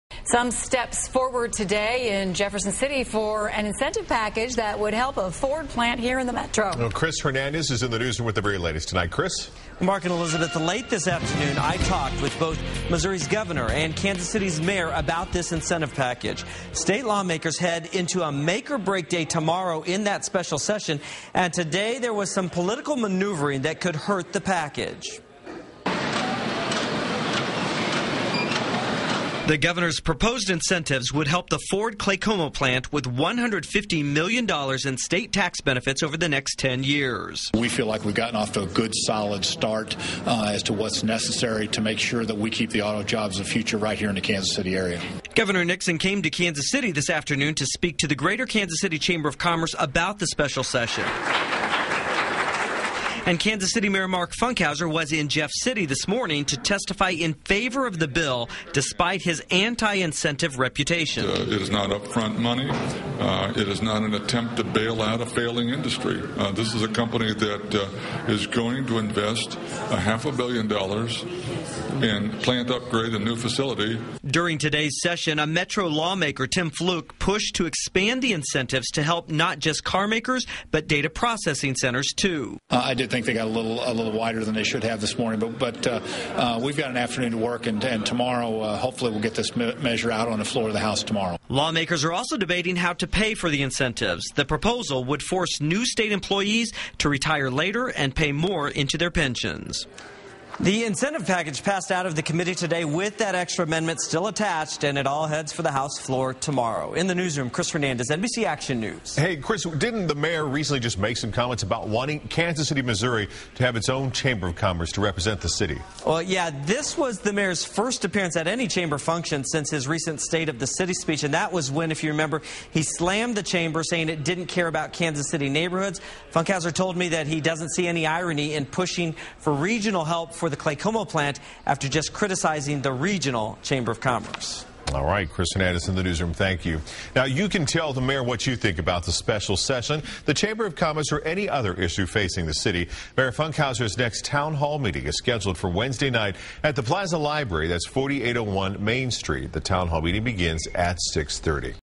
Add-ons could threaten Claycomo incentives
Incentives designed to keep the Ford plant in Claycomo pass out of committee in the Capitol, but lawmakers added an amendment that could spell trouble during debate Tuesday.